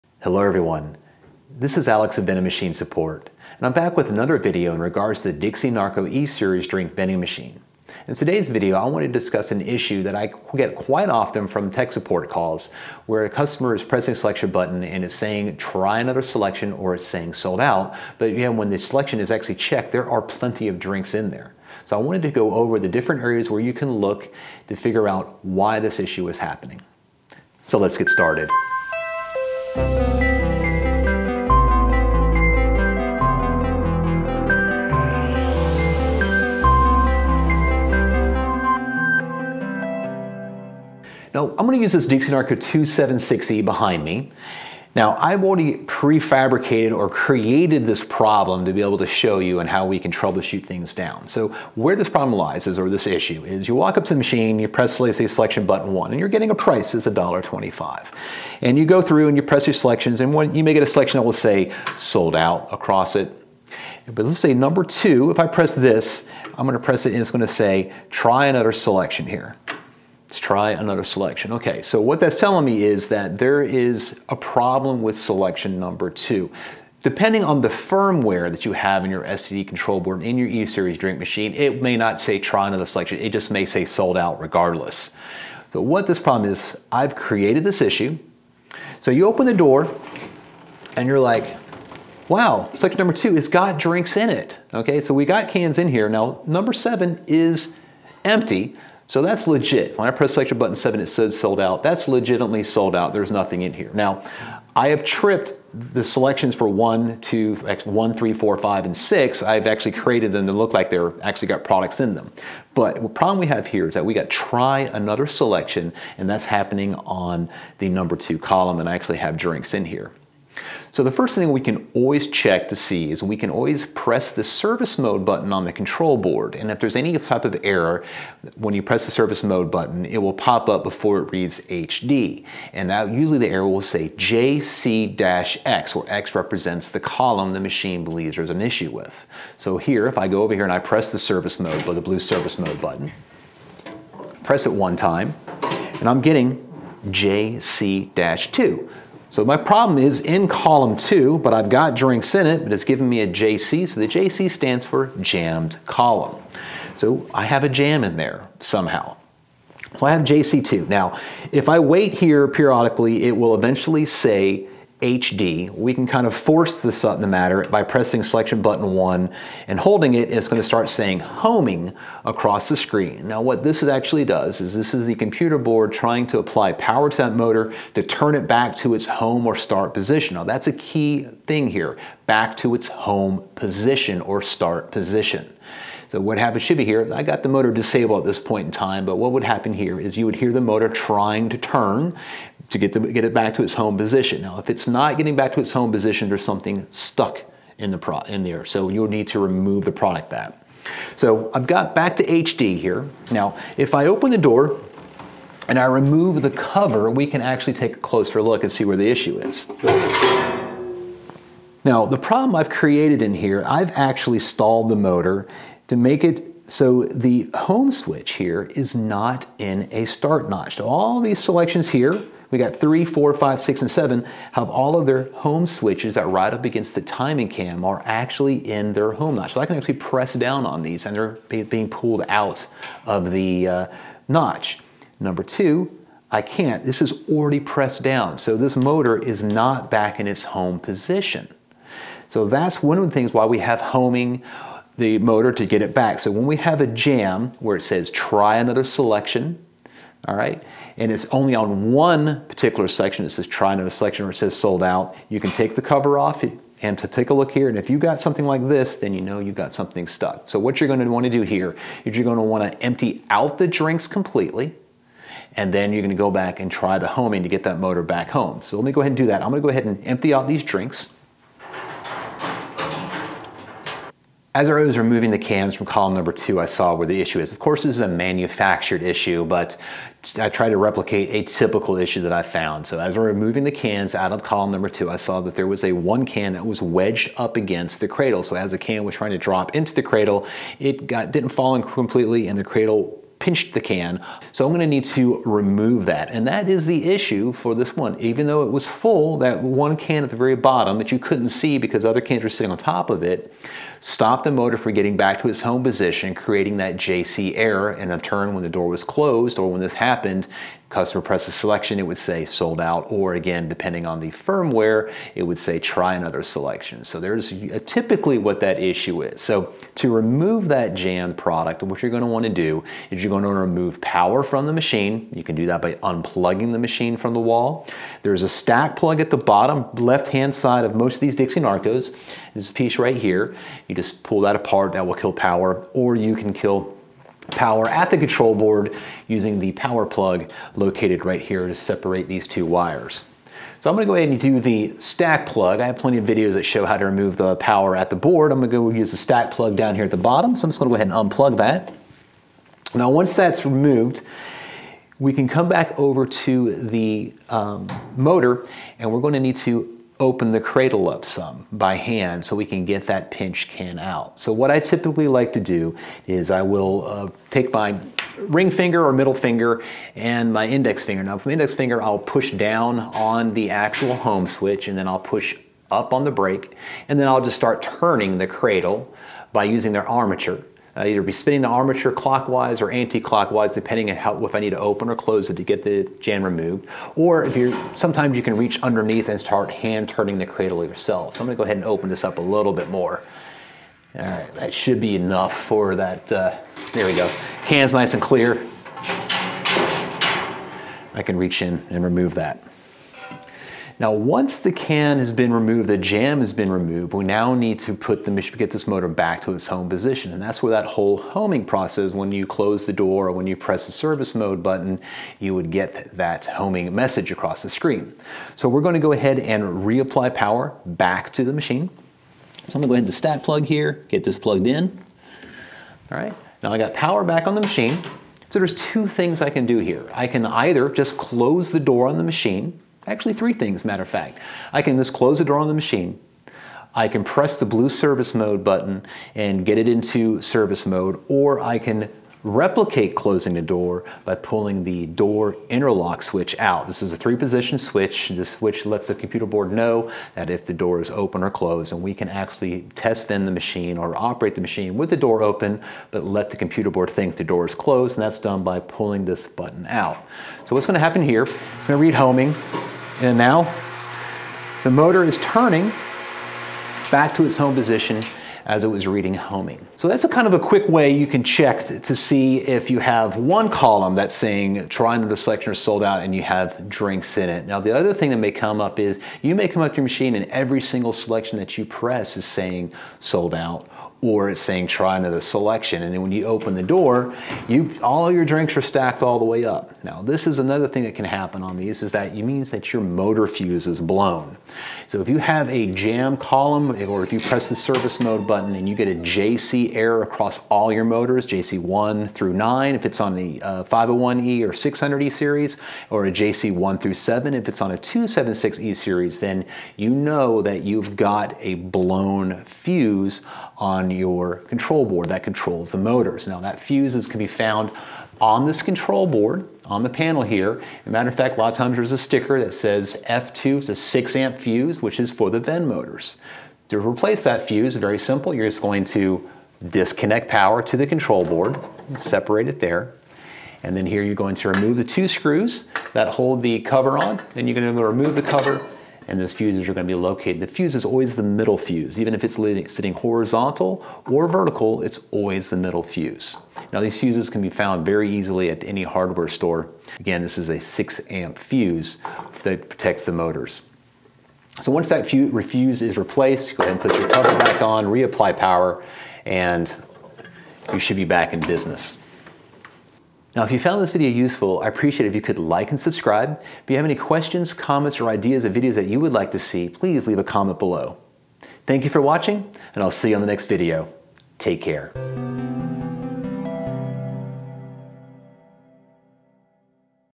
Dixie Narco Vending Machine: Jams & JC Errors
Have a 'Try Another Selection' or 'Sold Out' message on your Dixie Narco soda vending machine but there are plenty of drinks in the column? In this video, learn how to troubleshoot the JC error on your Dixie Narco drink vending machine like the 501e and 276e.

- Chapters -
00:00 - Intro
00:41 - Try Another Selection
02:01 - JC Error
02:50 - Vend Motor HOMING
03:32 - Vend Motor Home Position
04:45 - Jammed Can
05:48 - Vend Stack Plug
06:19 - Manually Removing The Jammed Can
07:23 - The Door Interlock Switch
08:31 - The Vend Motor Fuse
09:23 - Outro